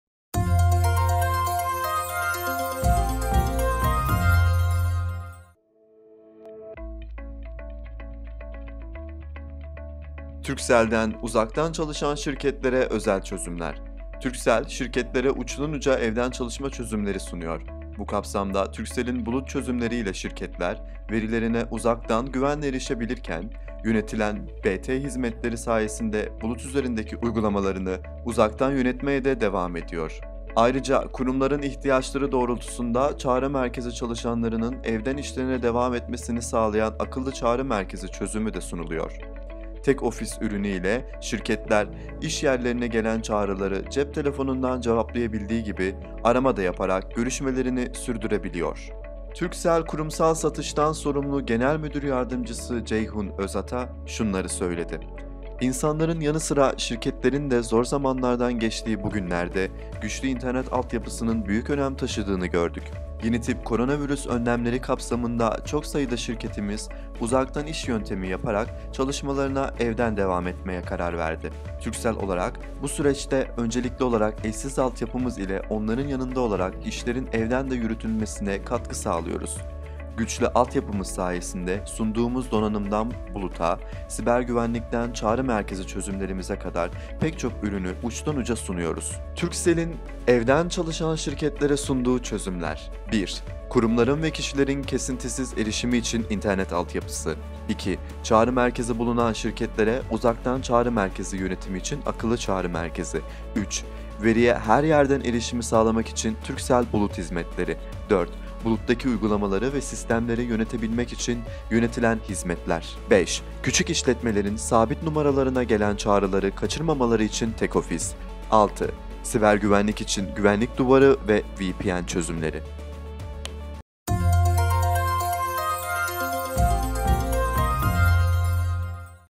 Turkcell’den uzaktan çalışan şirketlere özel çözümler
Turkcell, şirketlere uçtan uca evden çalışma çözümleri sunuyor. Bu kapsamda Turkcell’in bulut çözümleriyle şirketler, verilerine uzaktan güvenle erişebilirken; yönetilen BT hizmetleri sayesinde bulut üzerindeki uygulamalarını uzaktan yönetmeye de devam ediyor.